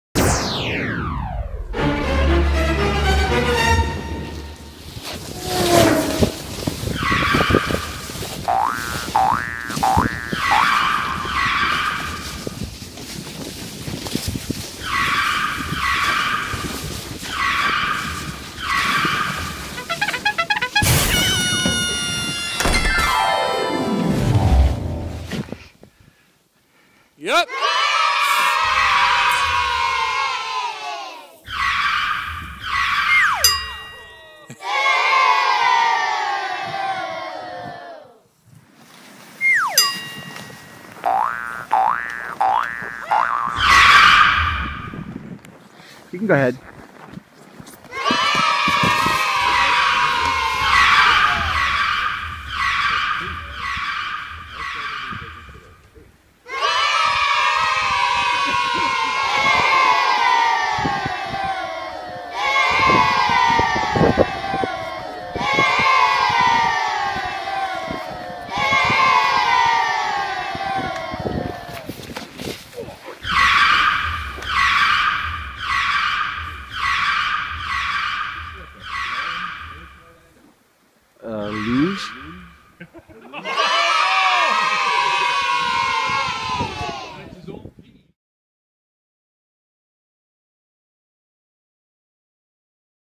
Sledding on Hobbs Hill
It's Tough Guy Weekend and once again sledding is a tough guy activity.